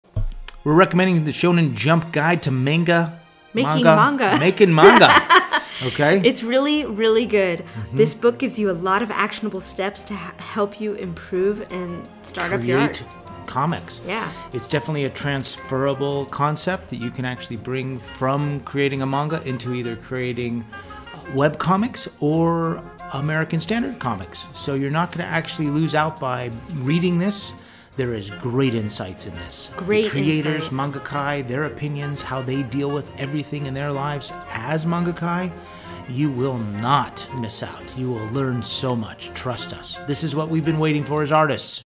A Great Guide to Making Comics & Manga!
The Shonen Jump Guide to Making Manga is the book we wished we had when we were just starting. Regardless of age this book has fantastic advice from professional manga creators.